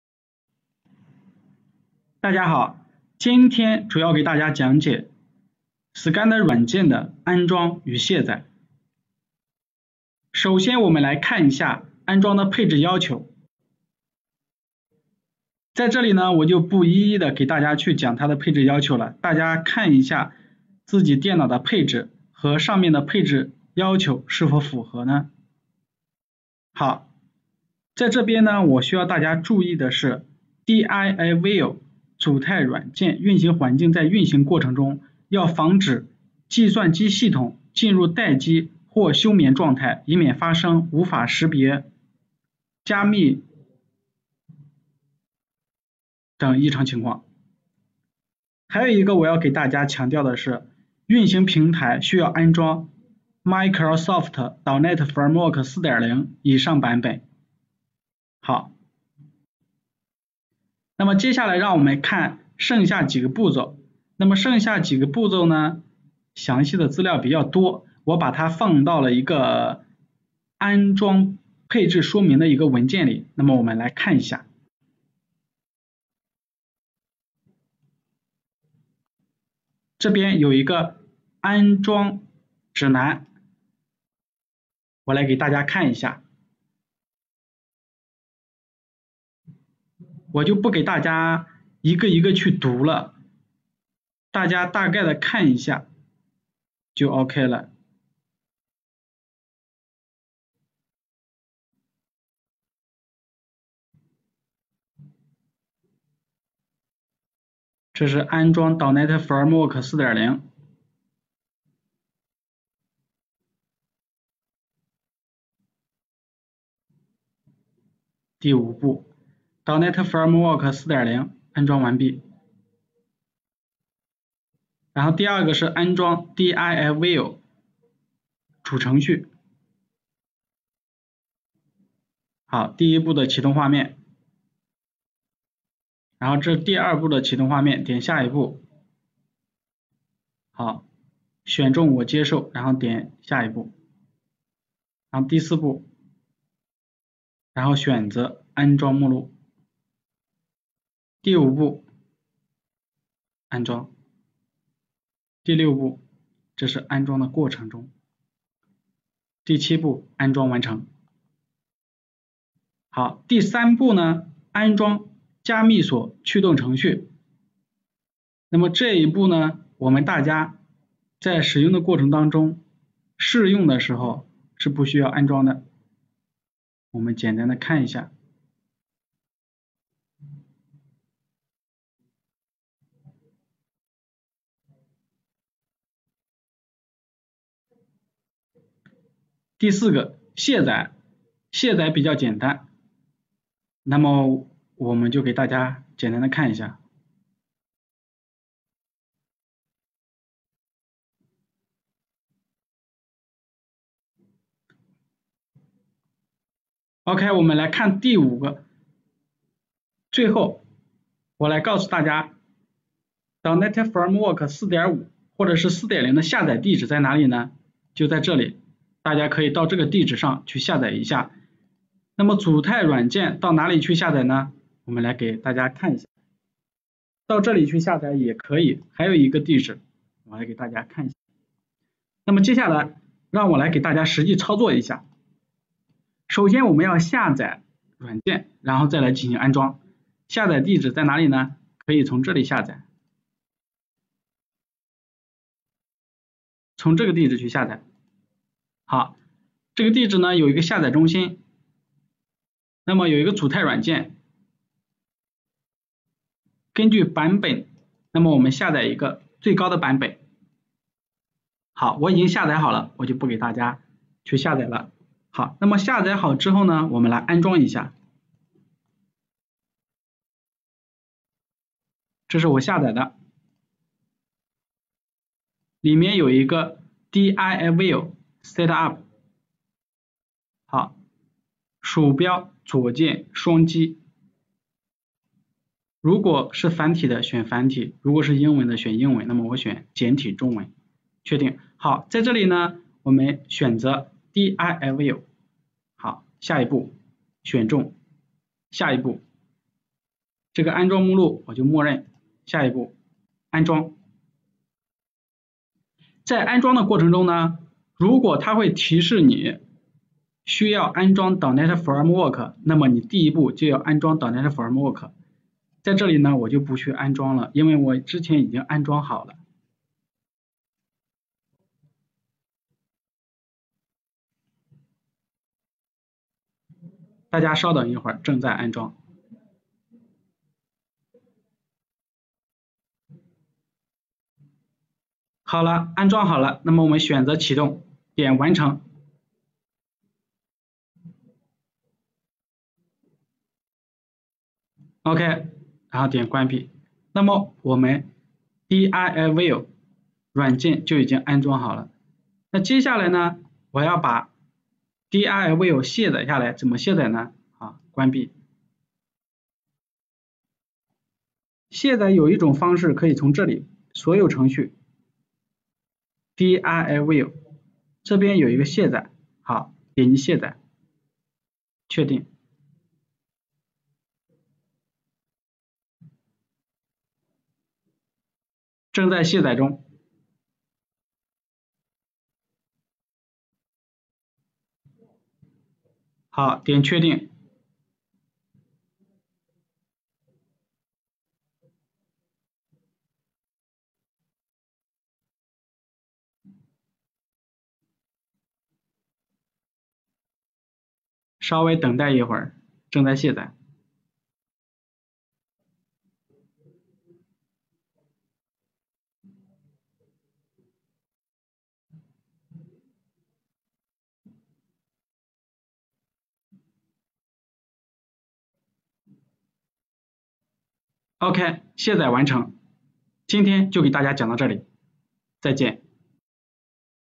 DIAView 08 DIAView SCADA Install
DIAView - Delta SCADA Software
About:

Ejecutar CMD como Administrador y ejecutar los siguientes comandos, si es que el Sistema Opertativo esta en Español:

net localgroup /add Users
net localgroup